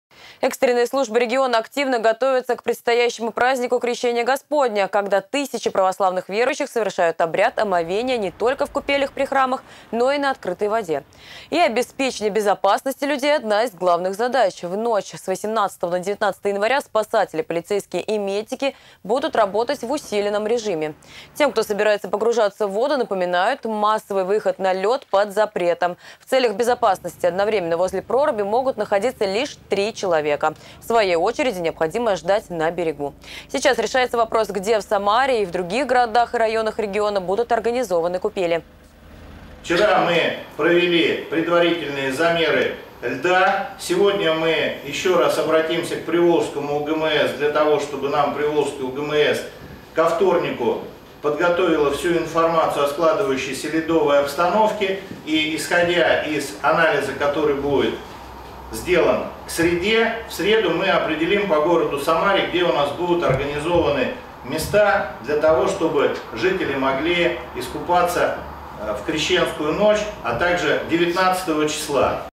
Спасатели в преддверии Крещения замерили толщину льда в Самарской области
Экстренные службы региона активно готовятся к предстоящему празднику  Крещения Господня, когда тысячи православных верующих совершают обряд омовения - не только в купелях при храмах, но и на открытой воде. И обеспечение безопасности людей - одна из главных задач. В ночь с 18 на 19 января спасатели, полицейские и медики будут работать в усиленном режиме. Тем, кто собирается погружаться в воду, напоминают, что массовый выход на лёд под запретом. В целях безопасности одновременно возле проруби могут находиться лишь три человека, своей очереди необходимо ждать на берегу. Сейчас решается вопрос, где в Самаре и других городах и районах региона будут организованы купели.